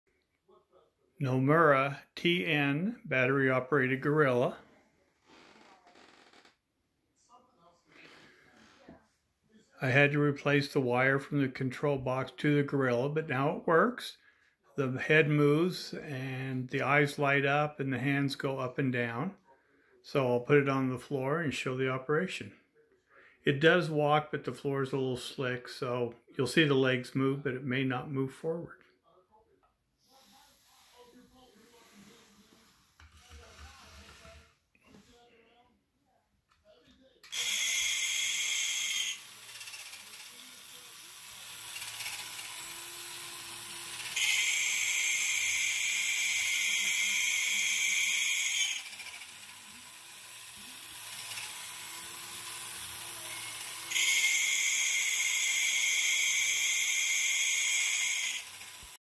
Gorilla
kingkong gorilla action vintage monkey giant. TN (Nomura)  battery powered remote control Gorilla. Cord from battery box to toy isn't original.